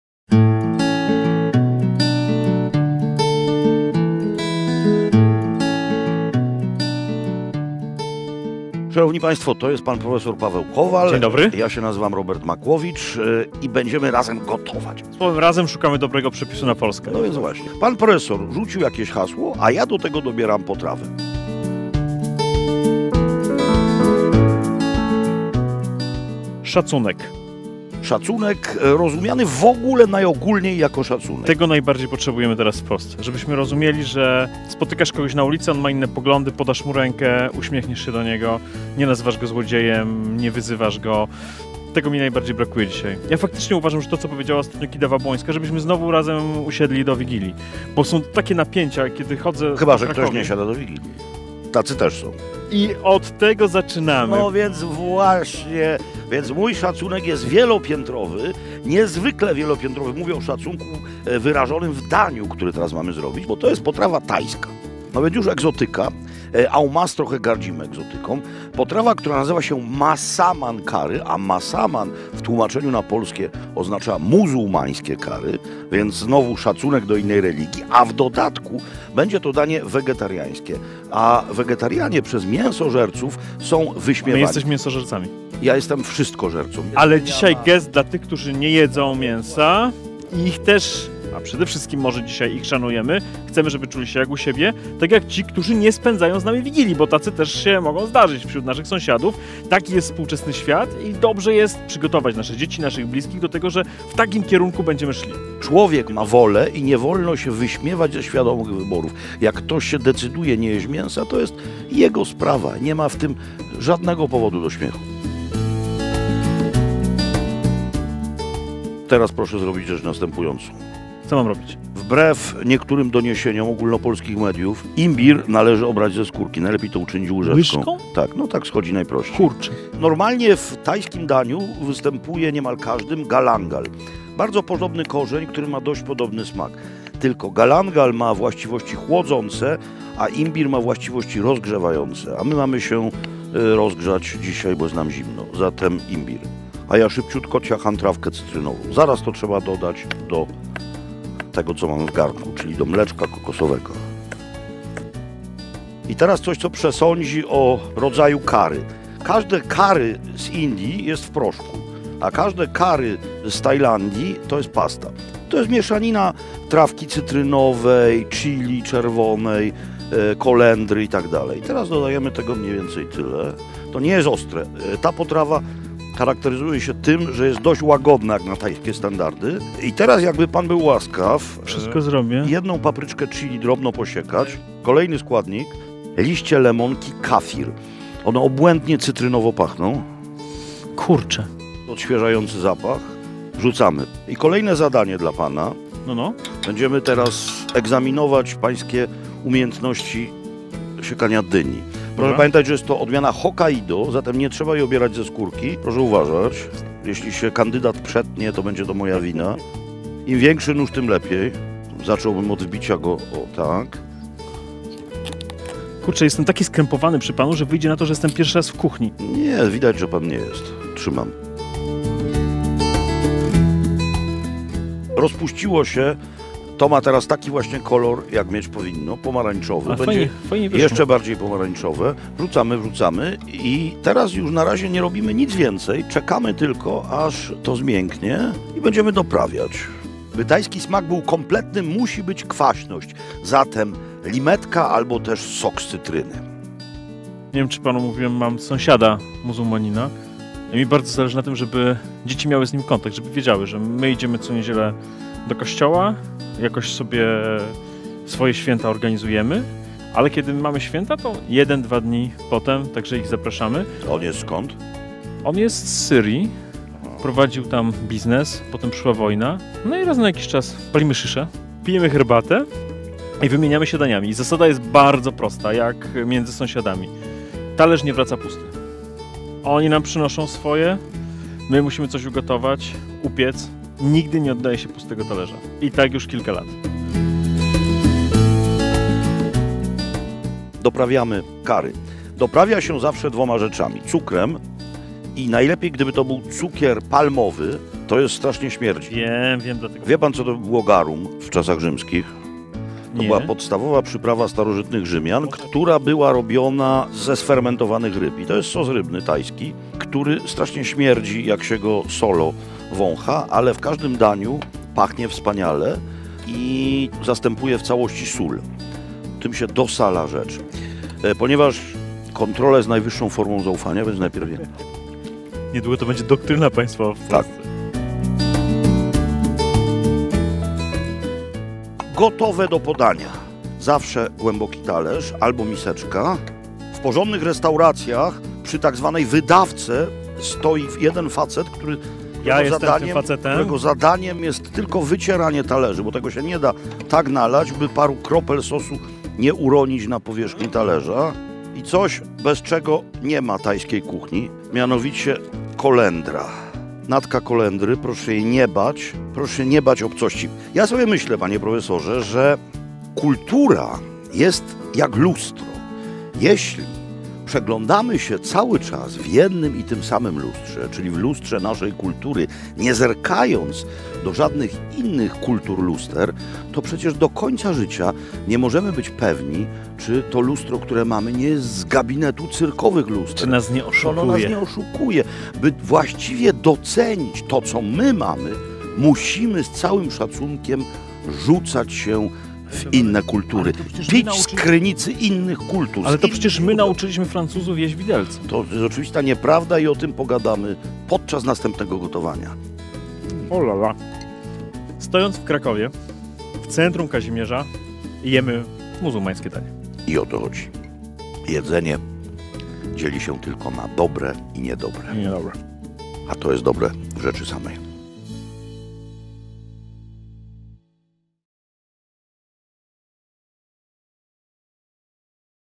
Paweł Kowal i Robert Makłowicz - Przepis na Polskę - Szacunek
Razem z Robertem Makłowiczem szukam dobrego przepisu na Polskę 🇵🇱